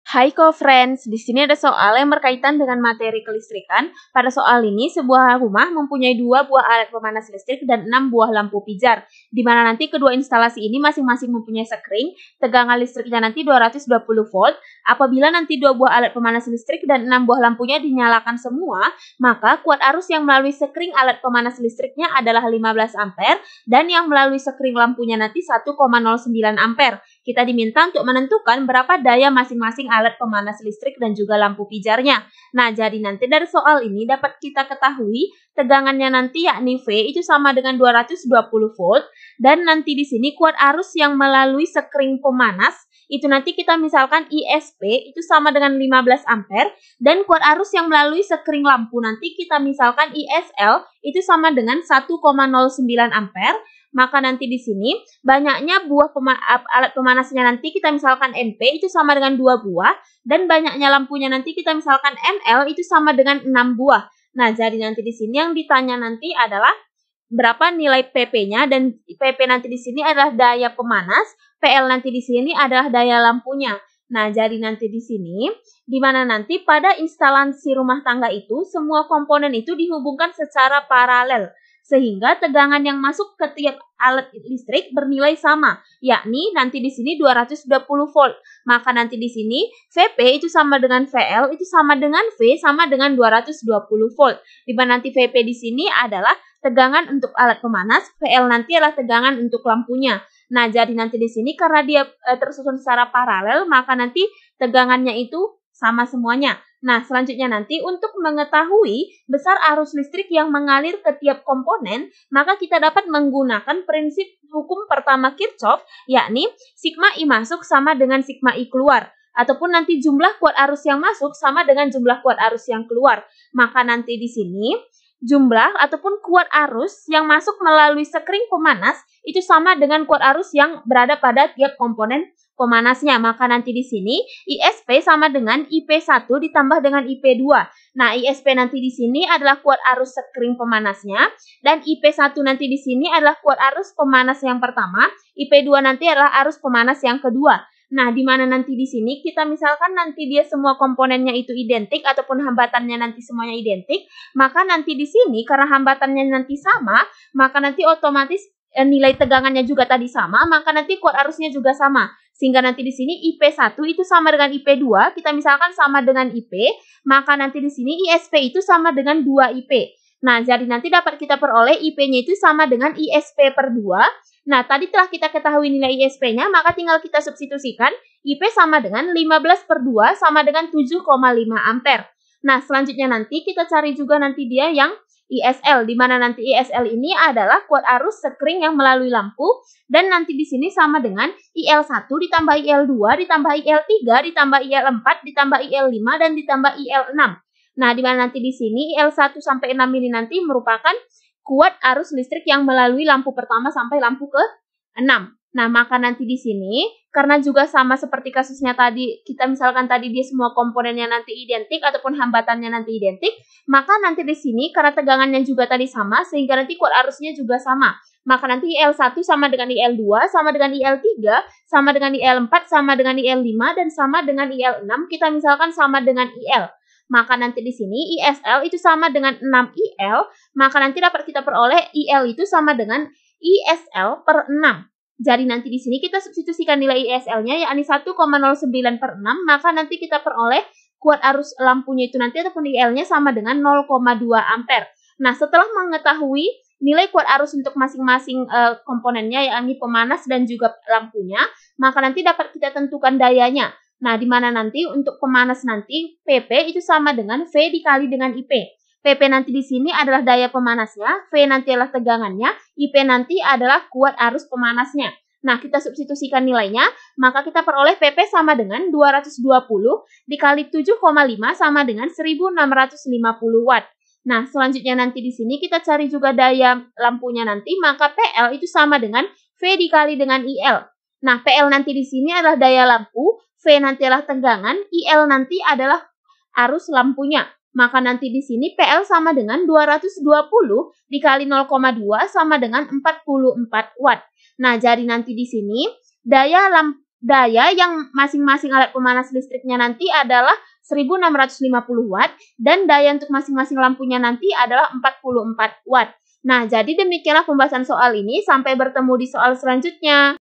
Sebuah rumah mempunyai 2buah alat pemanas listrik dan 6 buah lampu Kedua instalasi itu masing-mas...
Sebuah rumah mempunyai 2buah alat pemanas listrik dan 6 buah lampu Kedua instalasi itu masing-masing mempunyai sekering: Tegangan pijar: listriknya 220 V. Apabila dua buah alat pemanas listik dan enam buah lampu itu dinyalakan semua, maka kuat arus yang melalui sekering alat A dan yang melalui sekering lampu adalah pemanas listrik adalah 15 1,09 Berapa daya A masing-masing alat   pemanas   listrik   dan   lampu 0 pijar? hp 746 watt 1 =

Topik atau Materi: Sumber Energi dan Daya Listrik - Kelistrikan dan teknologi Listrik di Lingkungan - Elektro - Fisika Kelas 9 SMP - K-94

Sumber Soal: Soal-soal Fisika Untuk SLTP, K-94, WM, 1997 , Erlangga

00:00 : Bedah Soal
01:00 : Konsep, rumus dan pengertian Sumber Energi dan Daya Listrik
01:18 : Langkah penyelesaian soal
04:10 : Jawaban akhir
04:40 : Penutup